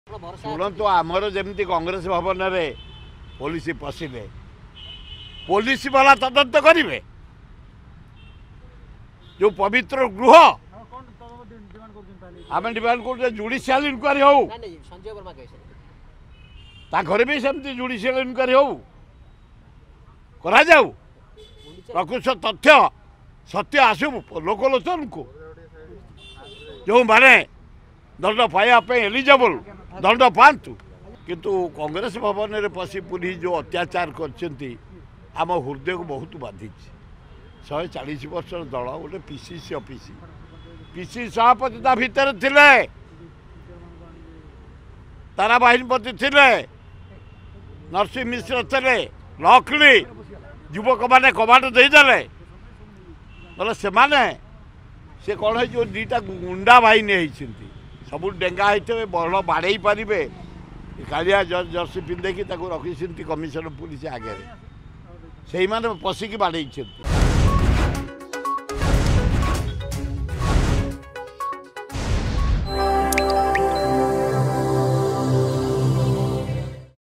କଂଗ୍ରେସ ଭବନରେ ପଶି ପୋଲିସ ଯେଉଁ ଅତ୍ୟାଚାର କରିଛି,ଆମ ହୃଦୟକୁ ବହୁତ ବାଧିଛି
କଂଗ୍ରେସ ଭବନରେ ପଶି ପୋଲିସ ଯେଉଁ ଅତ୍ୟାଚାର କରିଛି,ଆମ ହୃଦୟକୁ ବହୁତ ବାଧିଛି : ସୁର ରାଉତରାୟ,କଂଗ୍ରେସ ବିଧାୟକ